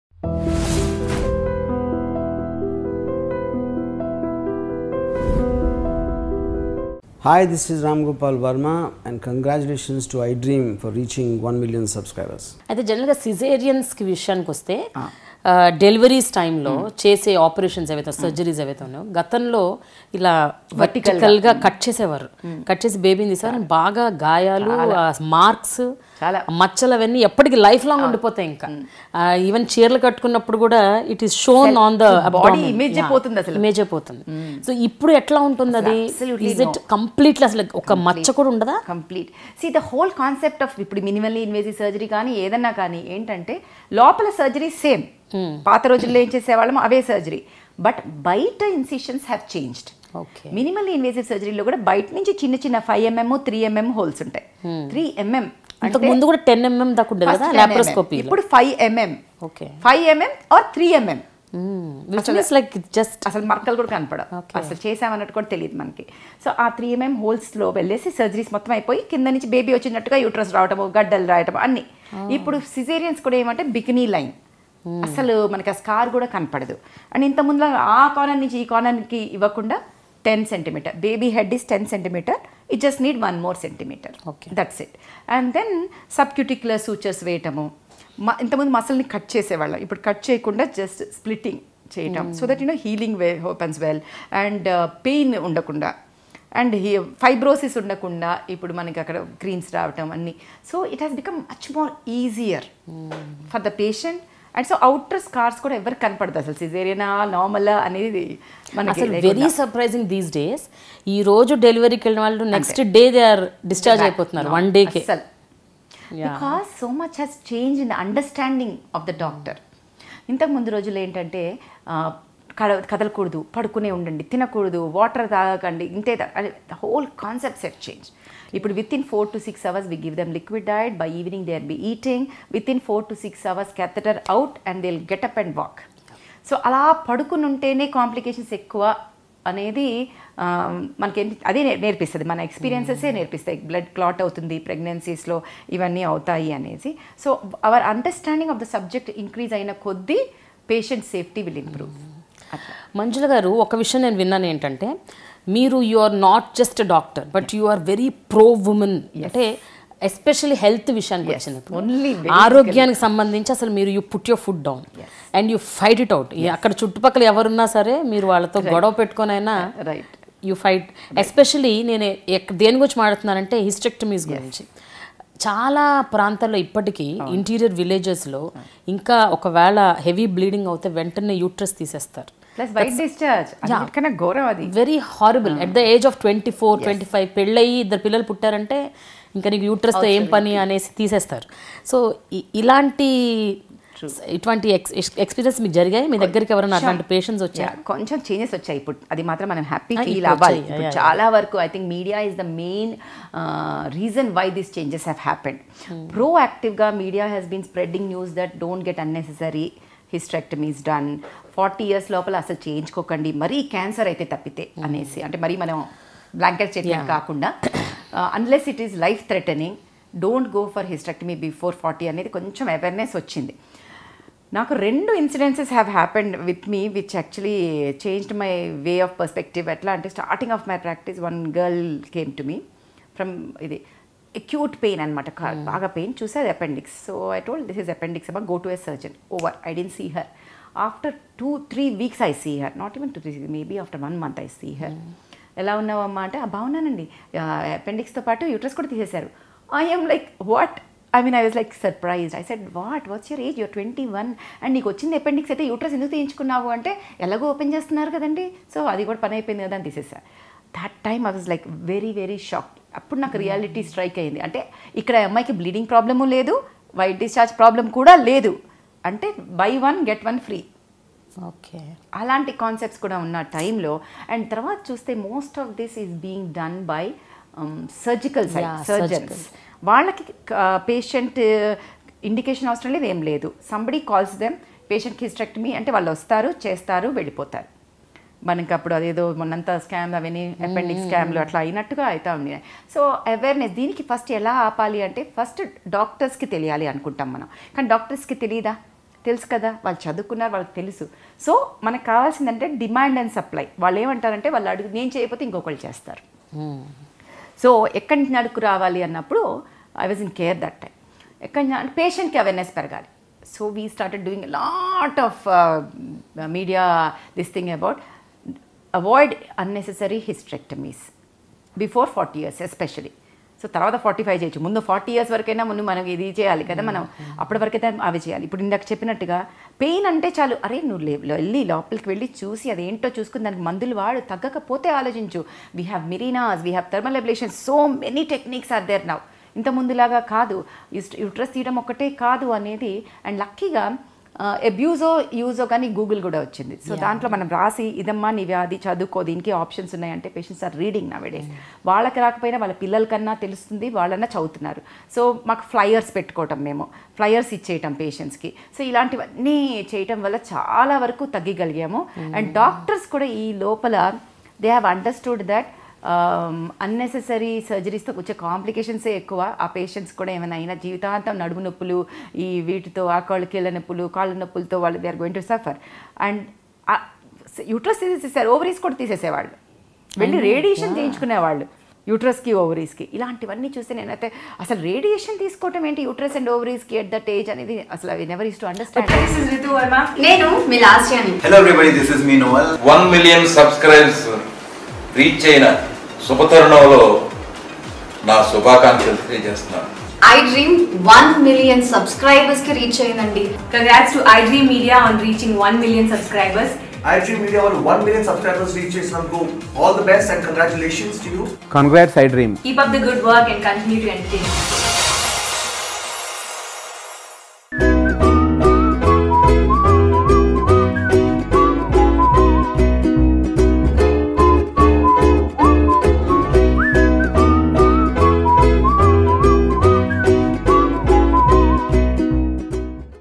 మీడియా వల్ల ప్రజల్లో చాలా మార్పు వచ్చింది - డా.మంజుల || Dialogue With Prema
Dr. Manjula Anagani, a well-known gynaecologist and obstetrician, saw the merits of the procedure especially in women’s health.  Manjula Anagani, MD, FICOG is the Head of the Department, Chief Gynaecologist, Obstetrician, Infertility Specility and Laparoscopic Surgeon at Maxcure Hospitals (old Sunshine Super Speciality Institute), Madhapur,Hitech City, Hyderbad. After nearly two decades of pursuing her passion and developing several new techniques in the field of laparoscopic surgery the doctor has been selected for one of the highest civilian honours, the Padma Shri. Manjula also received the prestigious Indian Affairs Indian of the Year 2016 – Medicine (Obs and Gynae) in Mumbai at Satya Brahma founded India Leadership Conclave 2016.

Manjula has completed her MD in obstetrics and gynecology from Osmania Medical College, following which she underwent training in prenatal genetic evaluation, infertility, ultrasonography and in minimally invasive procedures (hysteroscopy and laparoscopy). Dr Manjula Anagani pioneered the art of laparoscopy in late 90s when it was an emerging technology.

Her achievements include working on ergonomics of woman and evolving newer techniques in laparoscopy and minimal invasive surgery. She came up with a new entry technique to make the surgery less complicated and easier for the surgeon. Since then she has performed more than 10,000 laparoscopic surgeries in gynecology catapulting her at the top slot in the State and one among the few leading laparoscopic surgeons in India.